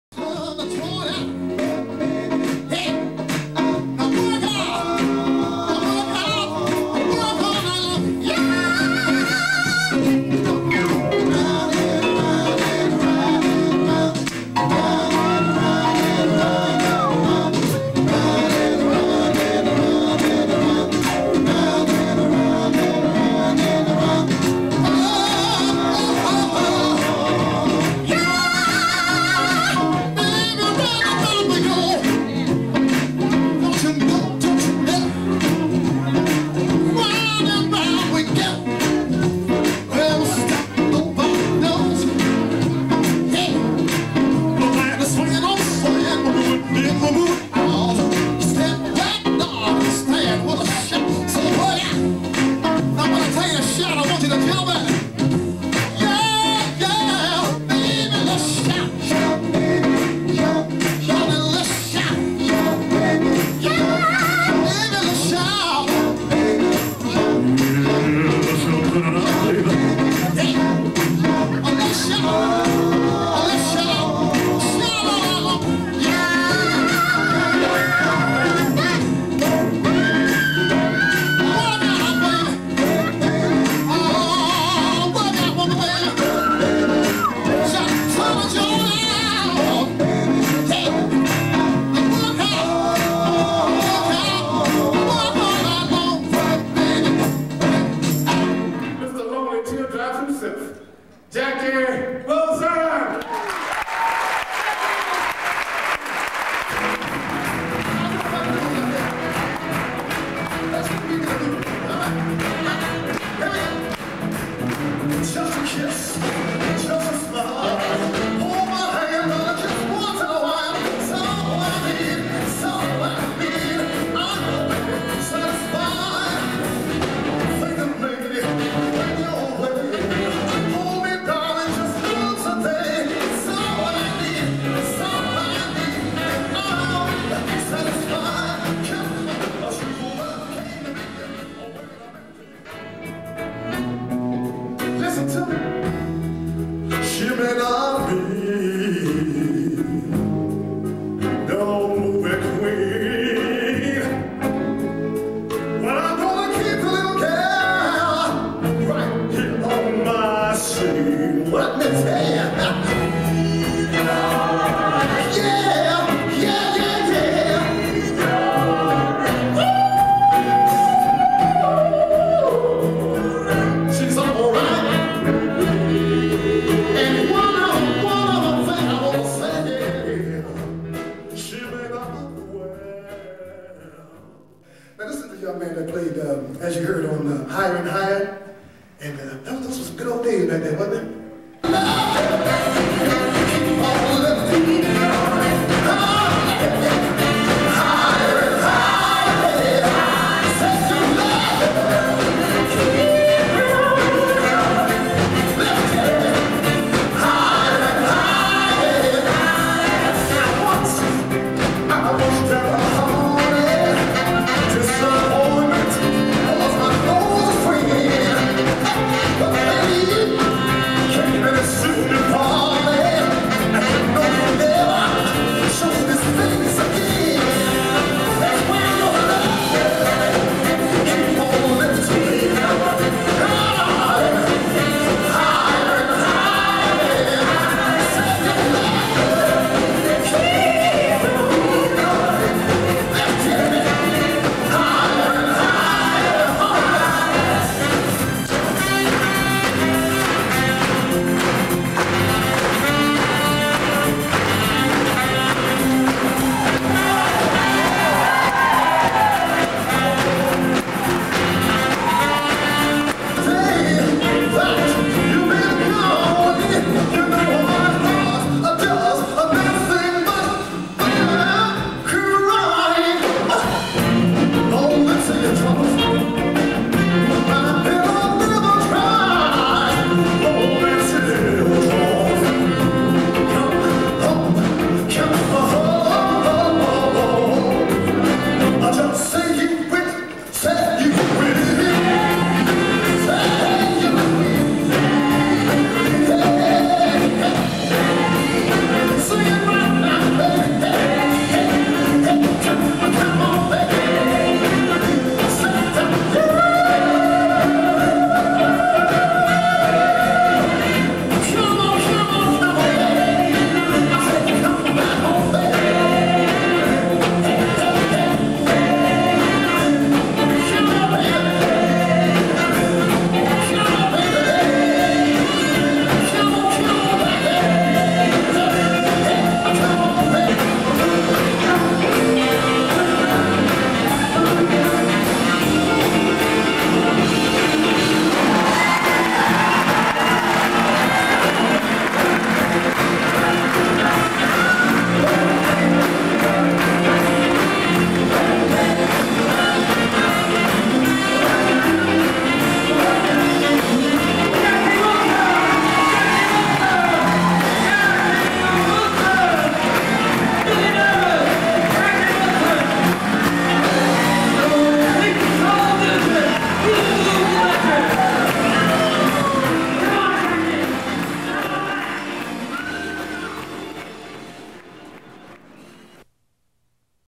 Pete Peterkin asThe Great JACKIE WILSON! Raw Footage//
Raw Footage///Not many entertainers other than JACKIE WILSON can claim to have been a major influence on JAMES BROWN, MICHAEL JACKSON and ELVIS PRESLEY.
Pete Peterkin;Raw footage of his portrayal of the man they called Mr. Excitement!